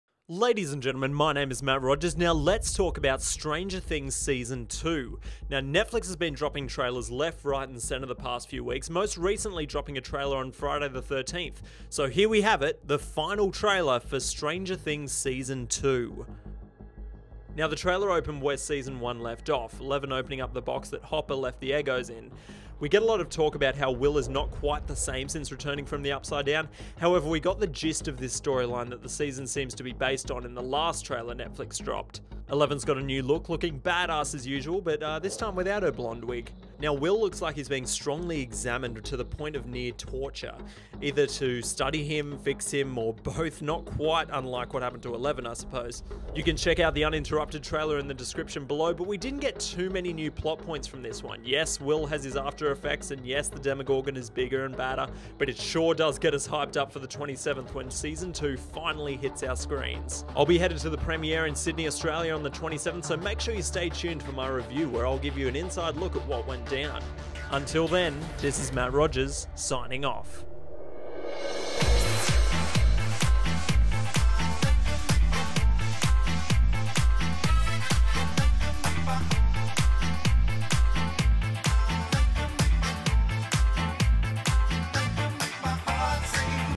STRANGER THINGS SEASON 2 | Final Trailer Discussion
Let's talk about the final trailer for Season 2 of the hit Netflix Original, "Stranger Things."